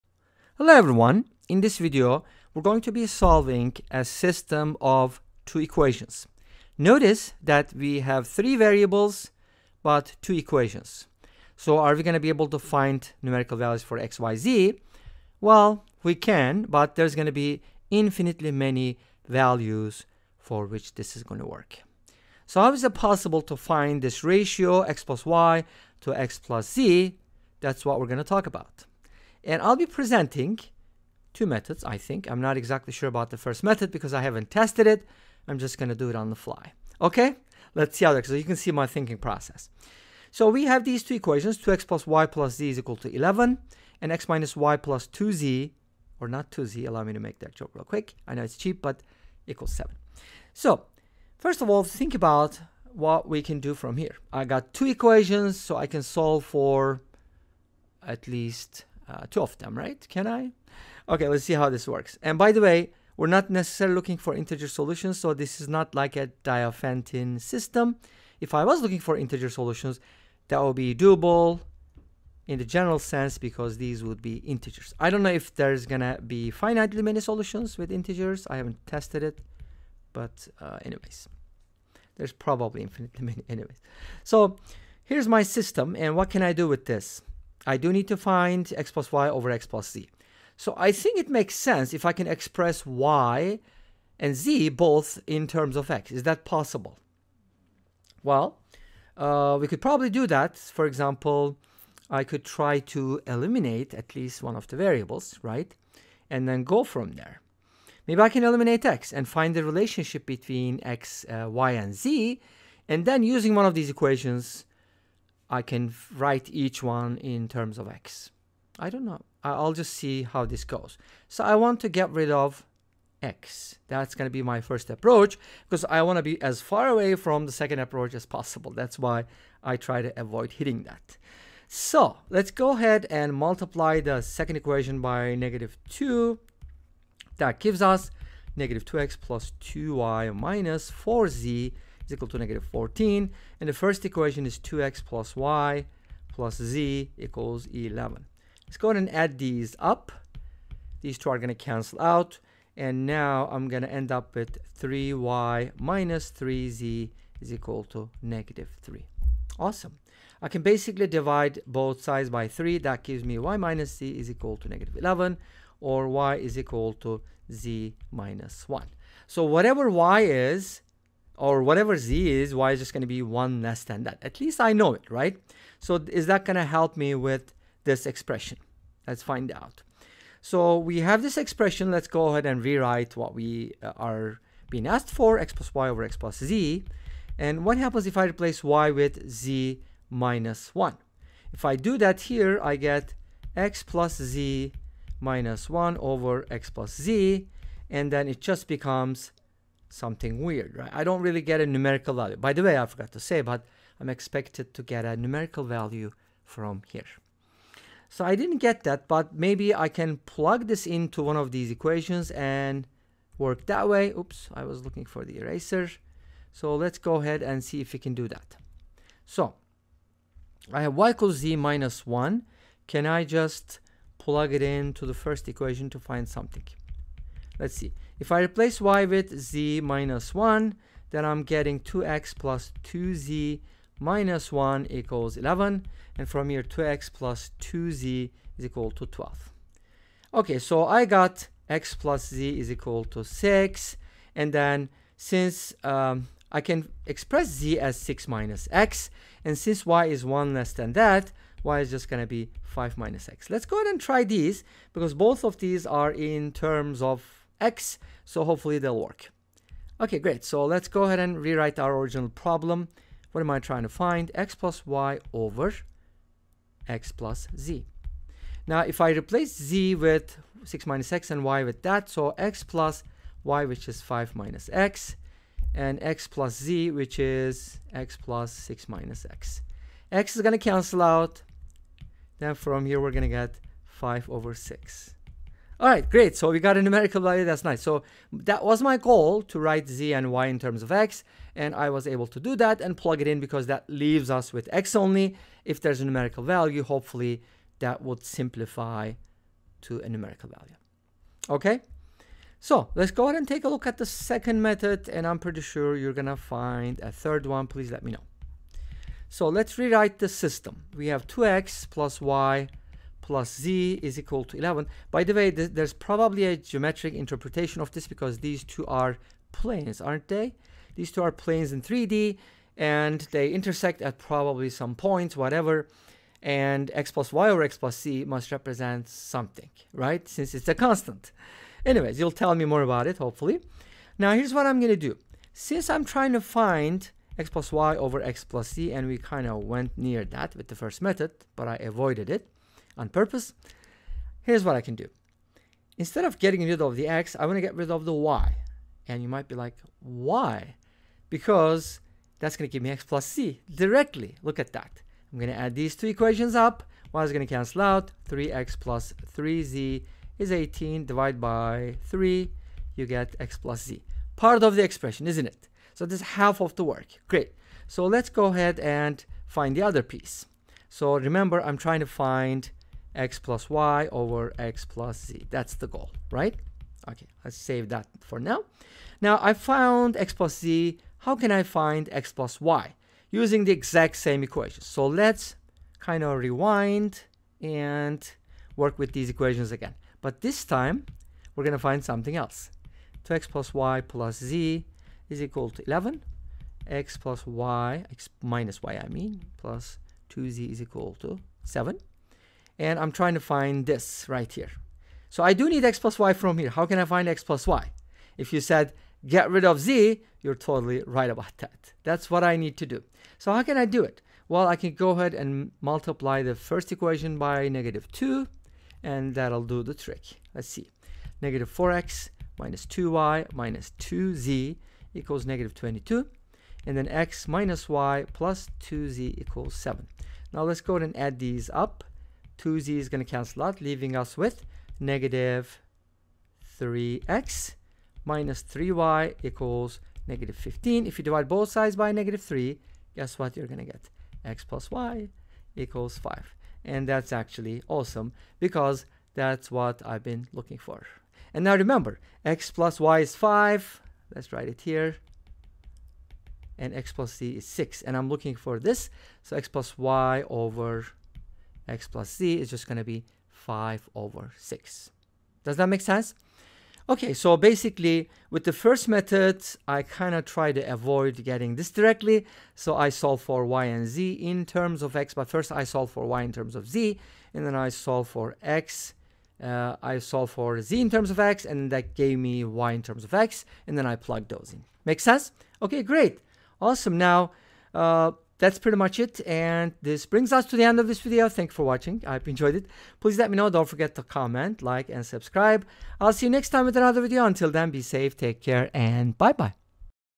A Linear System with Three Variables | Algebra Challenge
via @YouTube @Apple @Desmos @NotabilityApp @googledocs @canva
Linear System with two equations and three variables